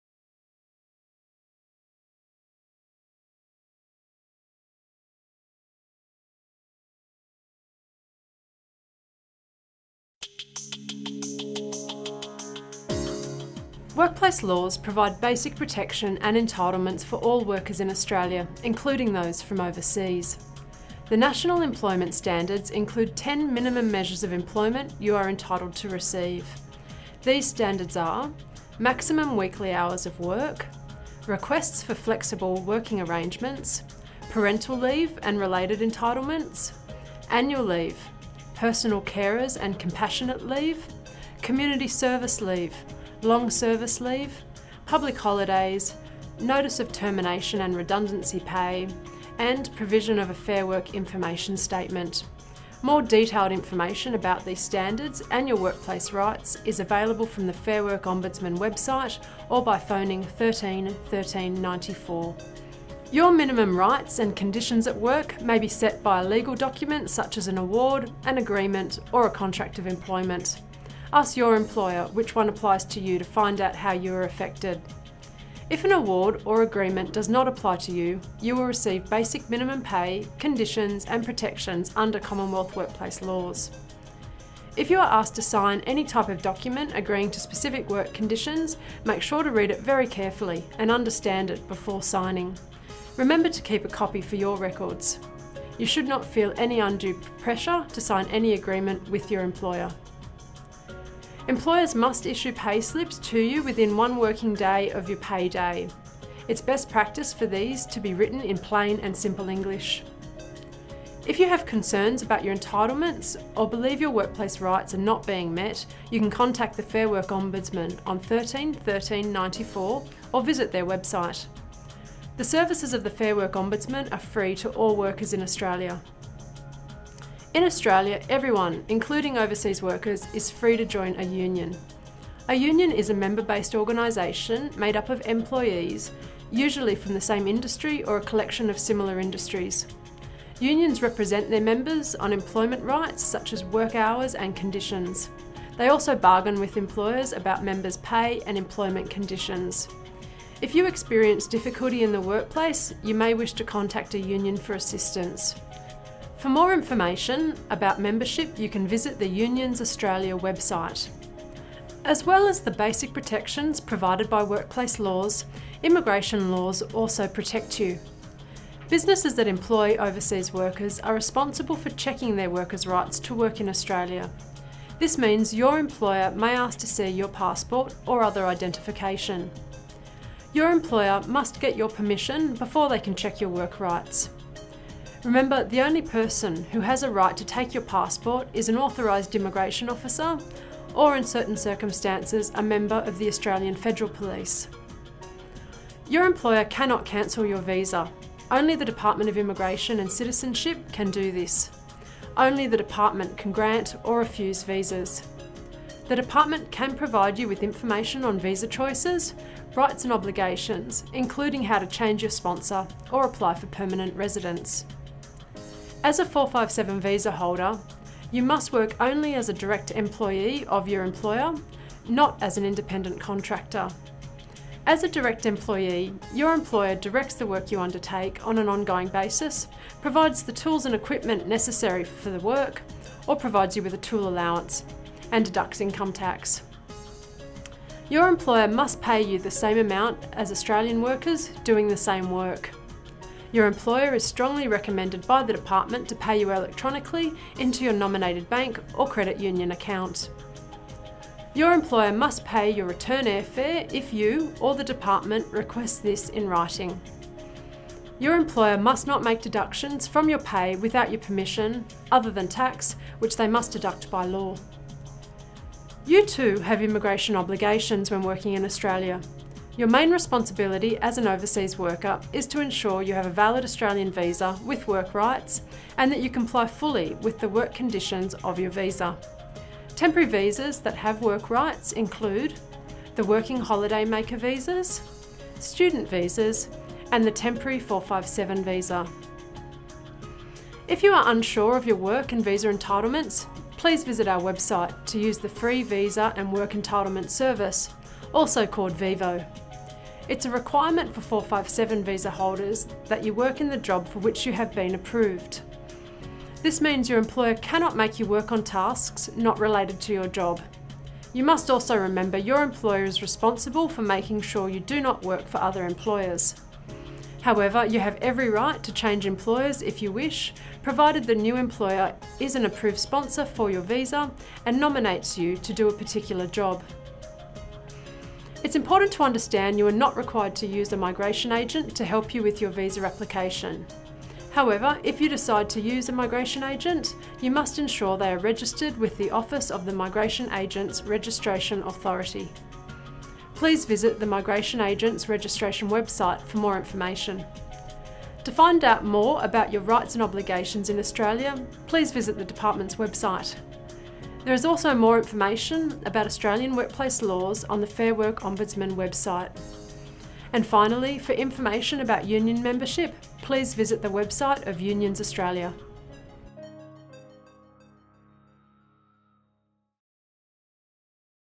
Your rights and obligations - Immigration facts for 457 visa holders
Workplace laws provide basic protection and entitlements for all workers in Australia, including workers who hold temporary visas such as the Temporary Work (Skilled) (Subclass 457) visa. As well as the basic protection provided by workplace laws, immigration laws also protect visa holders.

It is important if you are a temporary worker in Australia that you are aware of your rights and obligations.

More information about the rights and obligations for temporary resident workers is available on the department's

This video has been produced in consultation with the Fair Work Ombudsman, Fair Work Australia and Unions Australia.